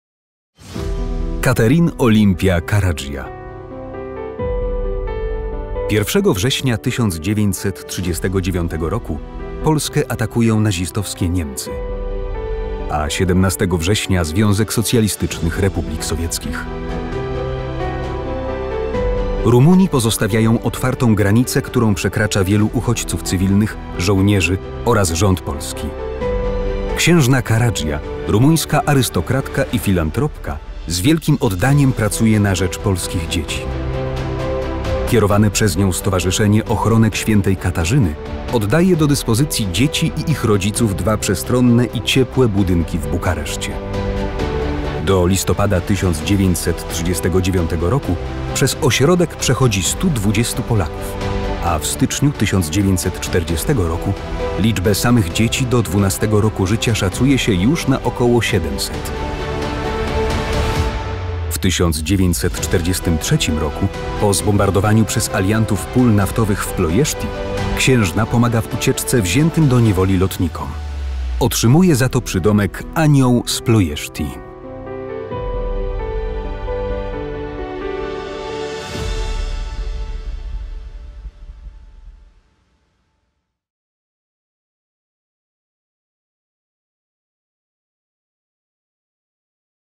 Ecaterina Olimpia Caradja | Virtus et Fraternitas
Wydawany w Bukareszcie „Kurier Polski” w numerze z 3 grudnia 1939 roku był pełen niepokojących nagłówków: „Atak Sowietów na Finlandię”, „Cierpienia Polski. Rozstrzeliwania i deportacje”. Jeden tytuł dawał jednak nadzieję: „Pod opieką ks. Caragea. Dom dla dzieci i matek”. W artykule tym opisano los dzieci polskich uchodźców w Rumunii – „małych, drobnych, niewinnych istotek, które w zaraniu życia spotkał ciężki los tułaczy”. Na szczęście „znalazły tkliwą opiekunkę”, czyli księżną Ecaterinę Olimpię Caradję.

Rumuńska arystokratka i filantropka prowadziła charytatywne Stowarzyszenie Ochronek św. Katarzyny. Już 24 września 1939 roku udostępniła polskim rodzinom dwa budynki w Bukareszcie, w tym przestronny pawilon. Dzieci miały tam do dyspozycji pokoje przedszkolne, salę zabaw i czytelnię. Najmłodsi podopieczni księżnej mogli liczyć na cztery posiłki dziennie, od razu uruchomiono też polską szkołę. Schronienie znalazło tam 120 Polaków. Jako przedstawicielka Międzynarodowego Komitetu Pomocy Dzieciom Caradja rozprowadzała również odzież dla polskich dzieci w innych skupiskach uchodźców.

Księżna często zabiegała u władz rumuńskich o większe wsparcie dla przybyszów z Polski. Współpracowała też z Paulem Superem, nadzorującym amerykańską pomoc dla przebywających w Rumunii Polaków. Część z nich jeszcze w czasie wojny wyjechała z Rumunii do innych krajów, pozostali w 1945 roku wrócili do Polski.

W 1952 roku Caradja musiała uciec z komunistycznej Rumunii do USA. Do ojczyzny wróciła dopiero pod koniec życia. Z racji udzielania alianckim jeńcom pomocy w dalszej części wojny zwana była na Zachodzie „Aniołem z Ploeszti”. „Aniołem” była też dla polskich dzieci. „Energiczna, wiecznie uśmiechnięta, z dobrym słowem dla każdego, wysłuchująca z wyrozumieniem skarg ludzi znękanych nieszczęściem” – tak pisano o niej w „Kurierze Polskim”.

Instytut Pileckiego służy upamiętnianiu, dokumentowaniu i badaniu polskiego doświadczenia i losów polskich obywateli w XX wieku.